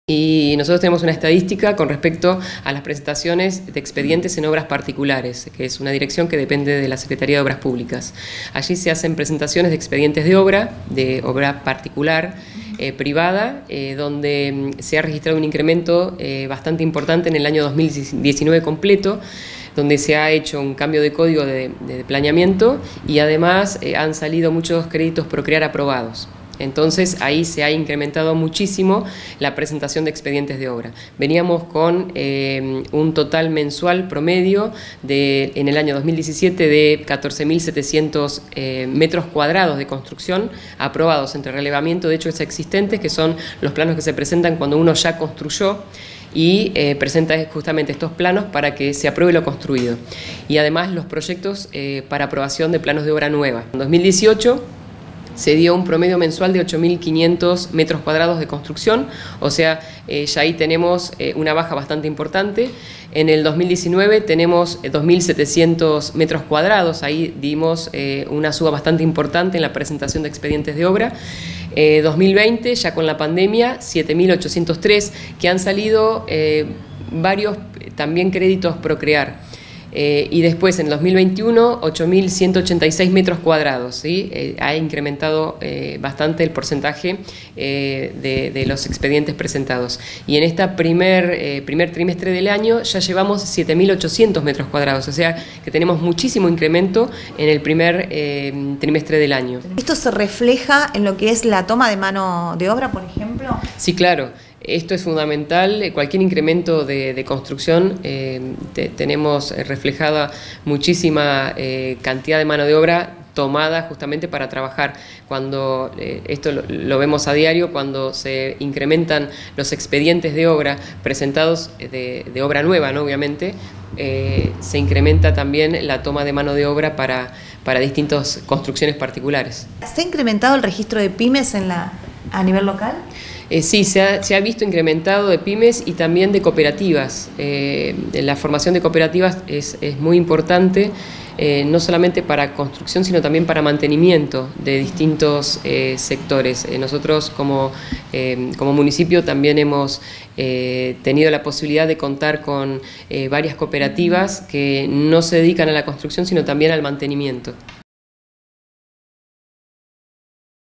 LuciaTaylor - "Las estadísicas son alentadoras"
La palabra de la secretaria de Obras Públicas de Puerto Madryn